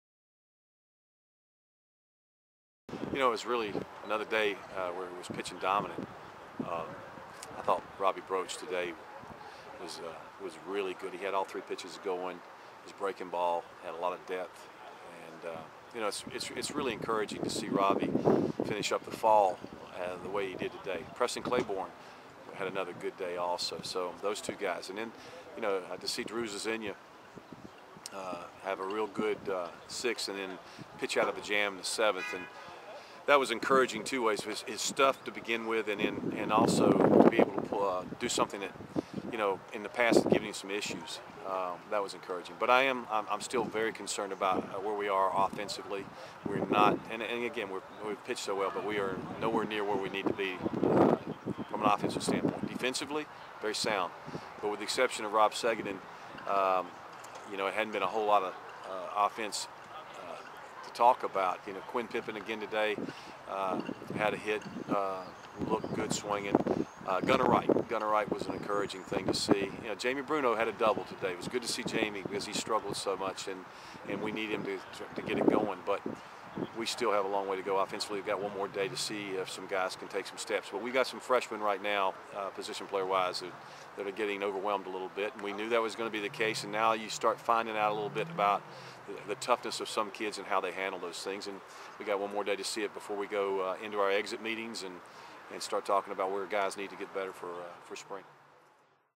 Fall World Series Game Four Recap
Tulane head coach Rick Jones discusses game four of the Intra-Squad Fall World Series